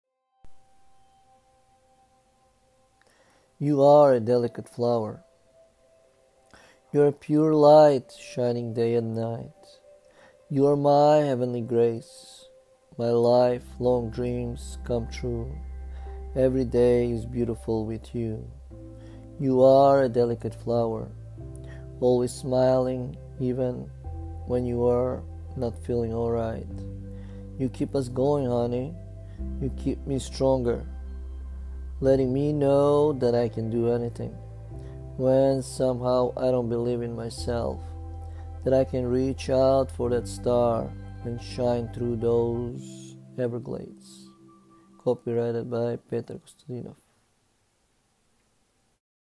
Petar Kostadinov- You Are A Delicate Flower(Spoken poetry)
You Are A Delicate Flower
You are a pure light
Shining day and night
You are my heavenly grace
My life long dreams come true
Every day is beautiful with you

always smiling even when you are
Not feeling alright

You keep us going honey
You keep me stronger
Letting me know that I can do anything
When somehow I don’t believe in myself
That I can reach out for that Star and shine through those everglades

(c)05/18/2021 by Petar Kostadinov